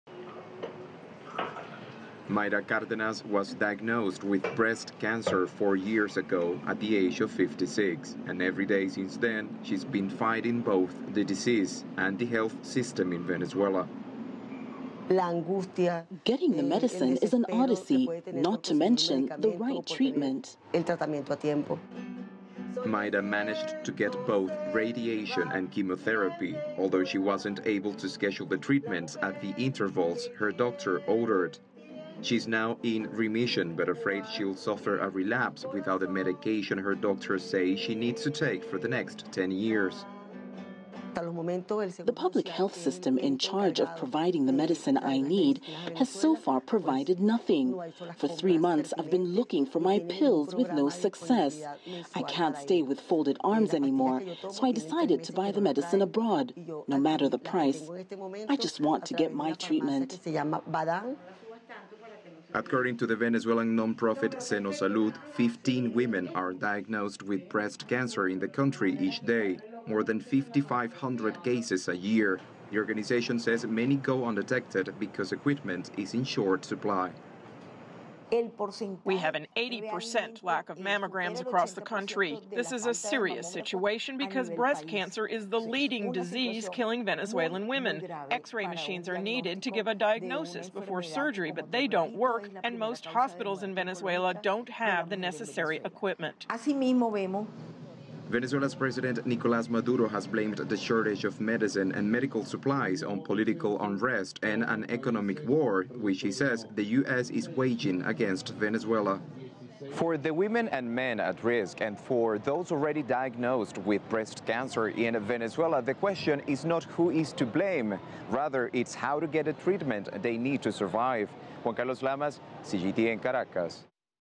Breast cancer patients miss treatments amid crisis in Venezuela
Venezuela's economic and political crisis has thrown many into desperation. Shortages of much needed products from food to medicine, are hurting some of the most vulnerable.

CGTN's Juan Carlos Lamas reports.